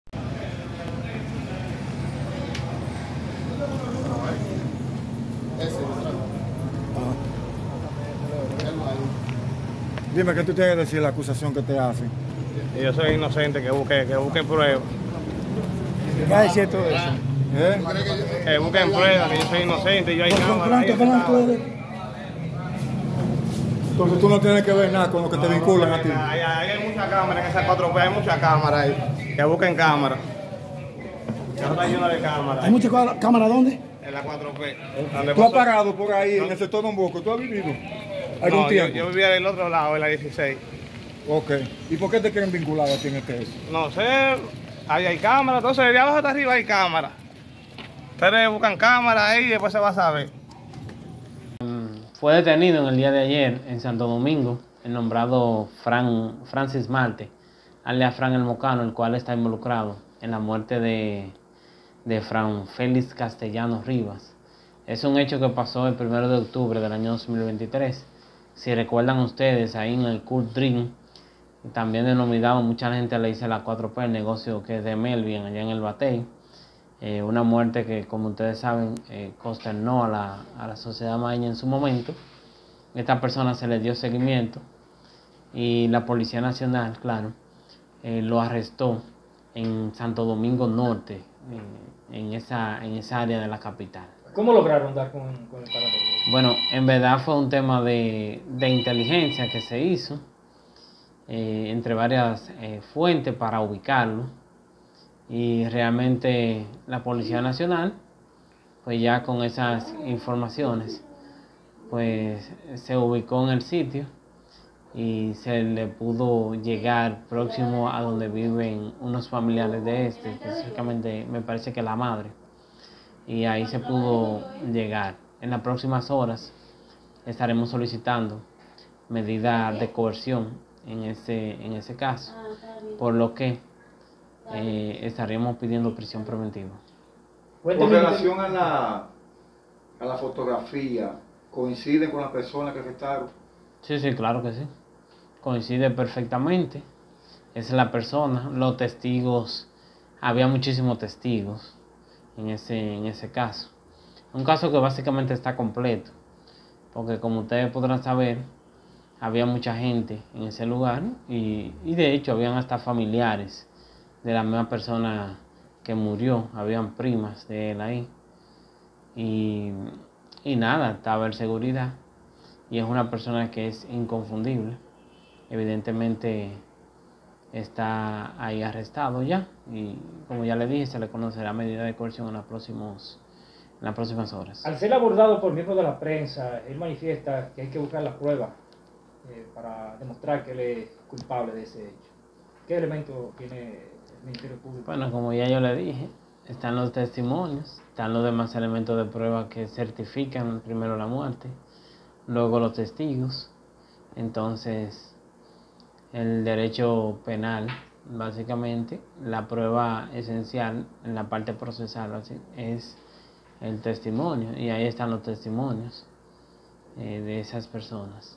Fue apresado el nombrado Frank, el mocano buscado por la justicia
Fue apresado el nombrado Frank, el mocano buscado por la justicia, acusado de darle muerte a otro joven en Mao.